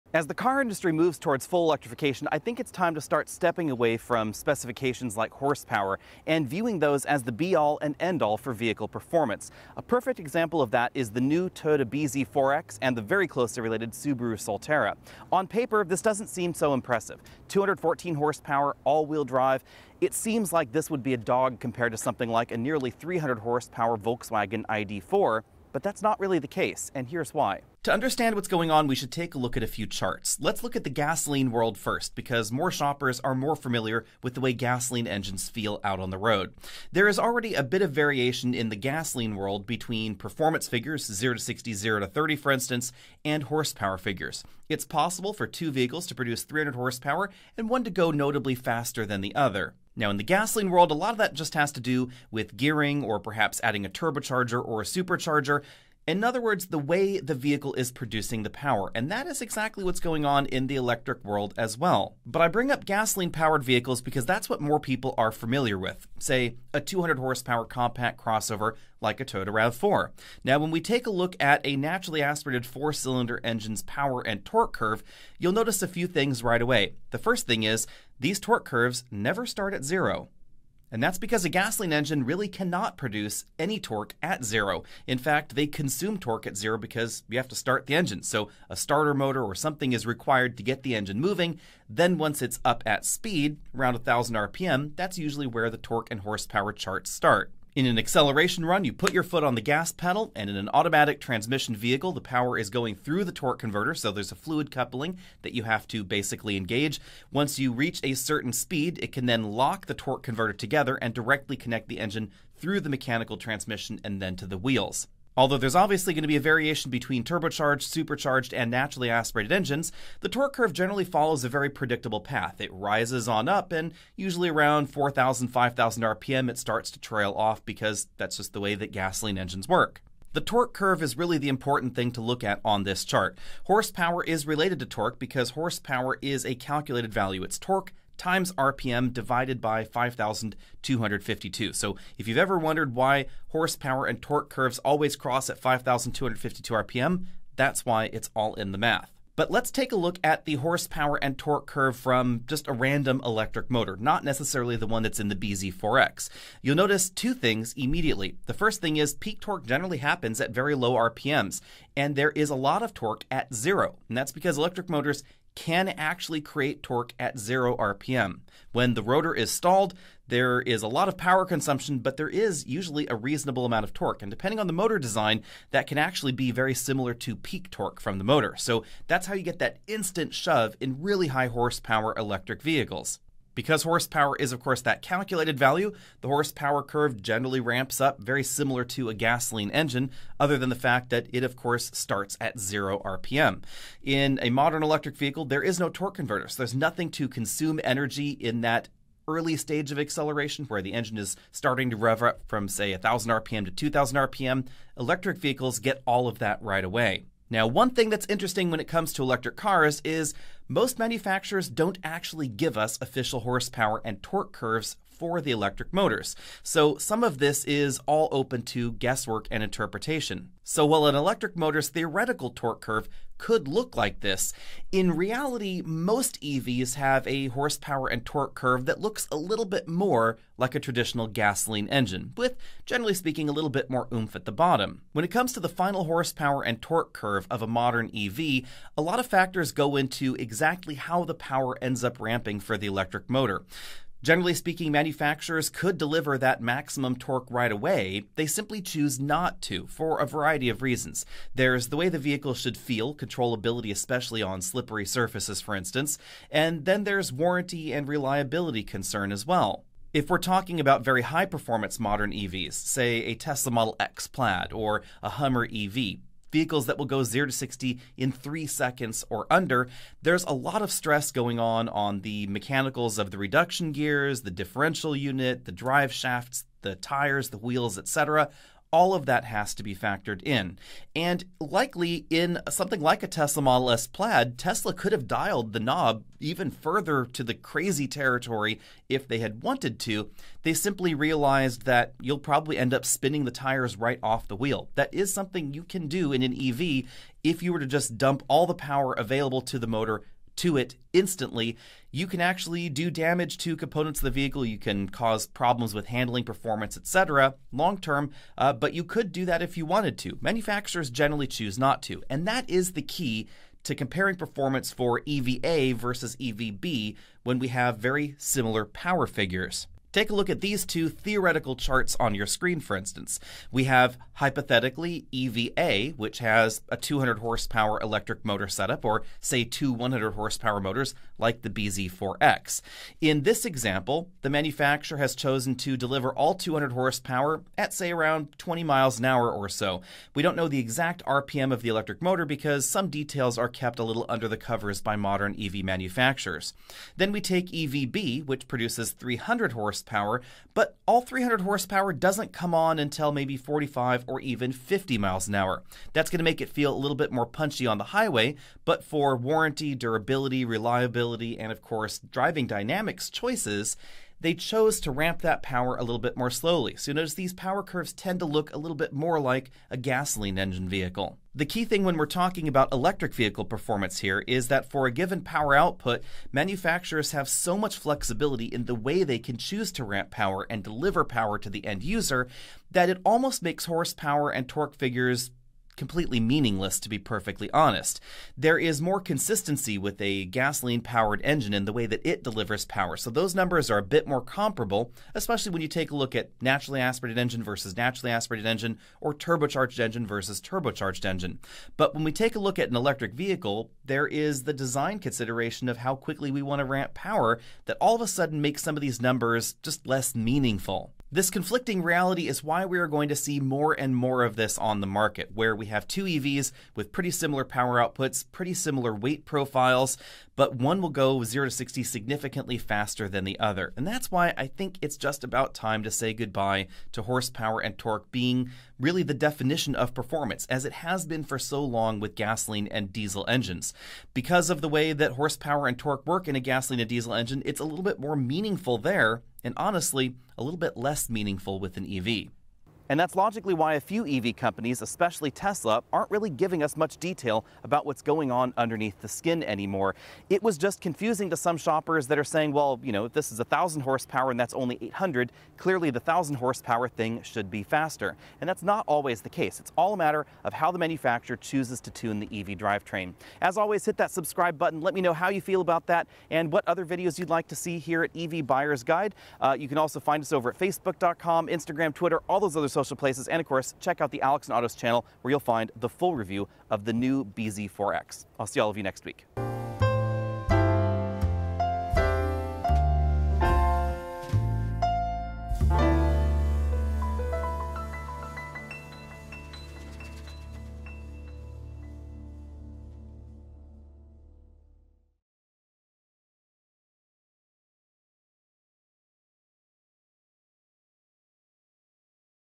With EVs, Horsepower Isn't Important Anymore
How can two cars get to 60 in almost the same amount of time despite wildly different horsepower specs? Because there's more to 0-60 than a spec sheet.